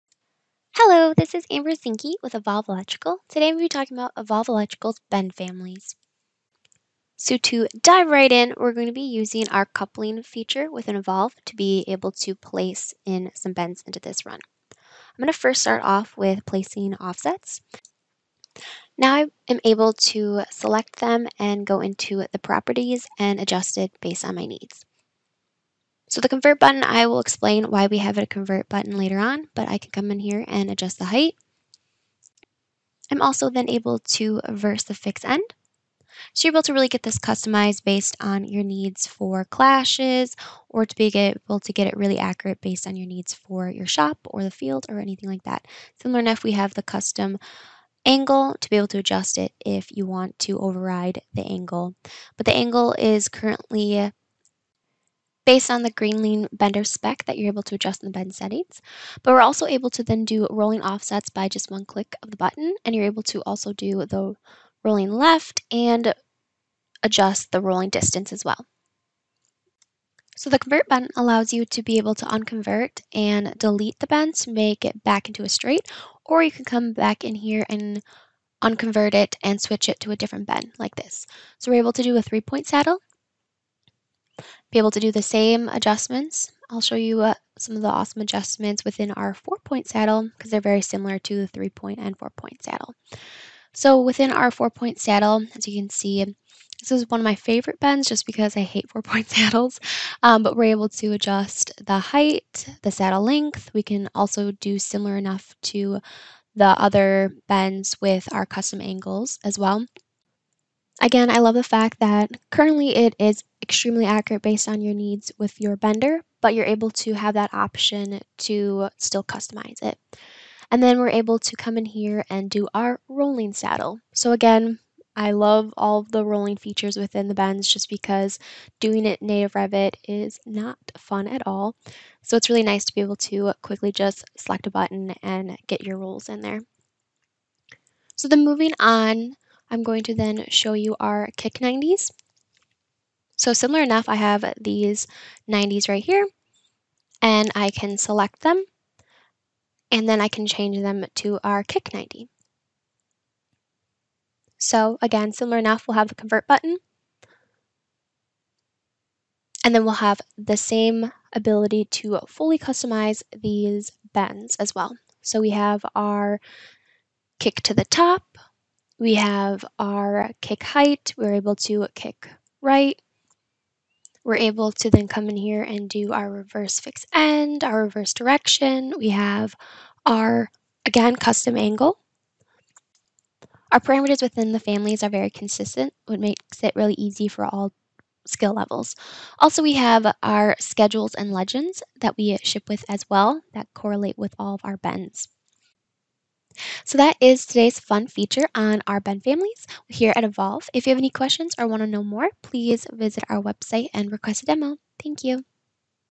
EVOLVE Electrical - Bend Families
The EVOLVE Electricals' bend families allow you to speed up your modeling time. You are able to fully customize the bend to fit your coordination needs, and they are easy to adjust when changes are needed. The bends look nice, are easy to use, and you can adjust the bender type to be accurate to what bender your company is using. Also, there are consistent parameters within the families that allow for easy scheduling.

▬ Social Media  ▬▬▬▬▬▬▬▬▬▬▬▬